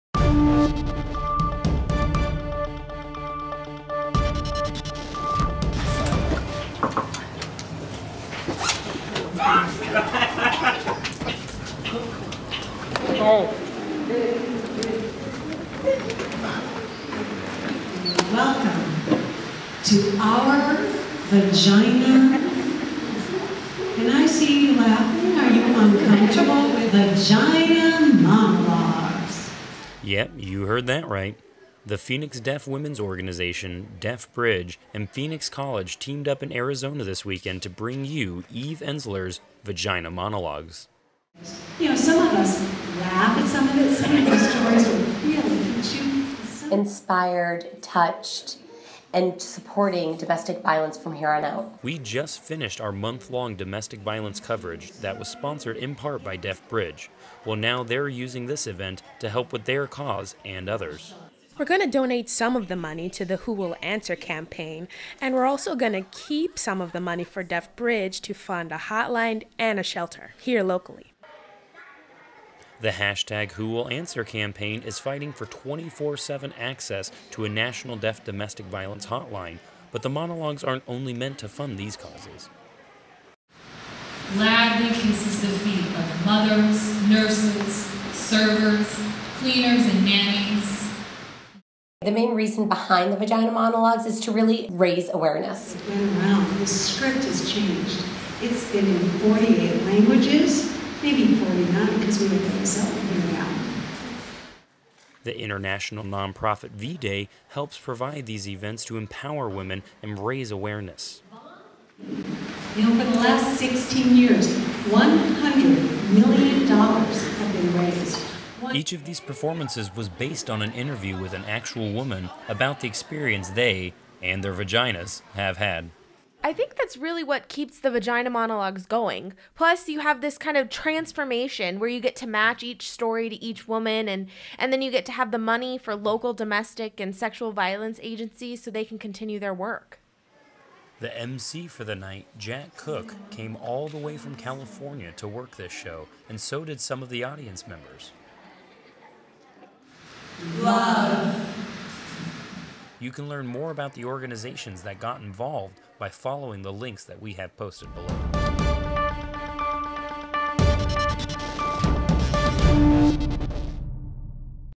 ASL Vagina Monologues Help Fund Local Women's and Domestic Violence Group
Phoenix hosts Vagina Monologues in ASL to raise money for Phoenix Deaf Women Organization and DeafBridge. People from all over came out in support to "empower women" and "support domestic violence groups."